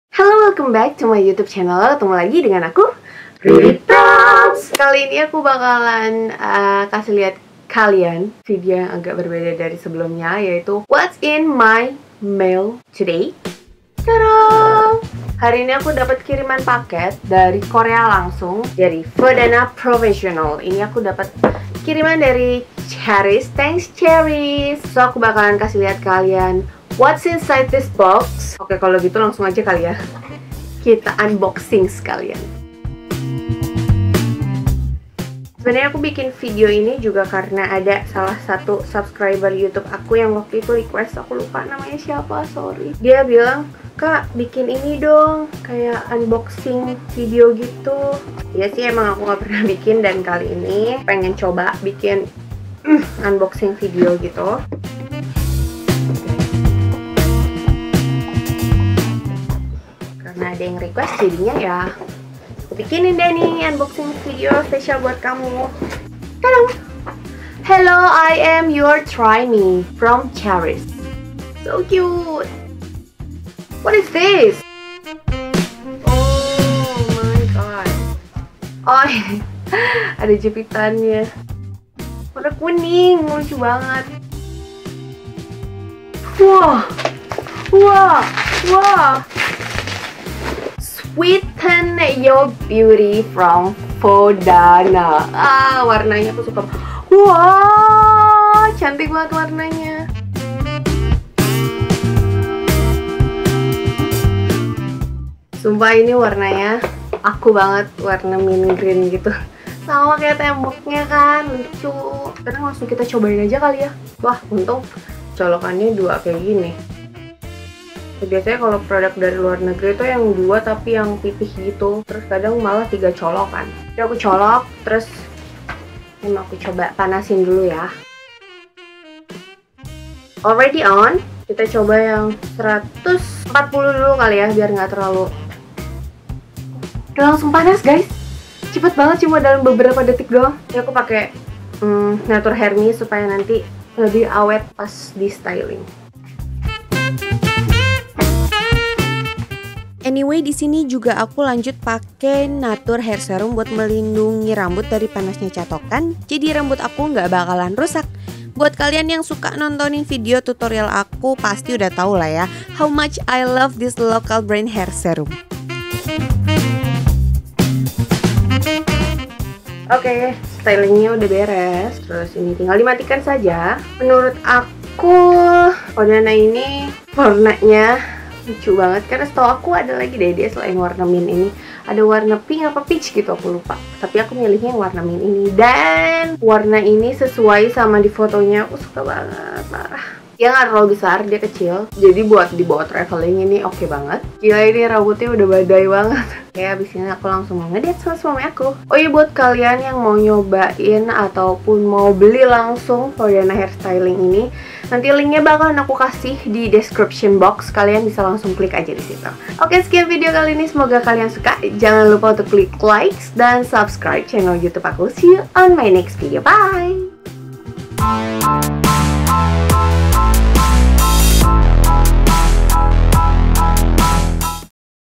Unboxing Catokan Dari Korea [BAHASA] | Ririeprams
Kali ini aku mau bikin segment baru nih di youtube channel aku, walaupun mungkin gak akan sering-sering juga sih share ginian. Tapi sepertinya sesekali boleh lah di coba, nama segmentnya adalah WHAT'S IN MY MAIL TODAY? Dan aku bakalan unboxing beberapa paket yang aku terima dan langsung aku cobain saat itu juga di depan kalian langsung hehe.

Thanks ya buat kalian yang udah request video unboxing ini. Semoga kalian suka!

Buat yang mau beli catokan ini, bisa langsung mampir ke sini (dikirim langsung dari Korea)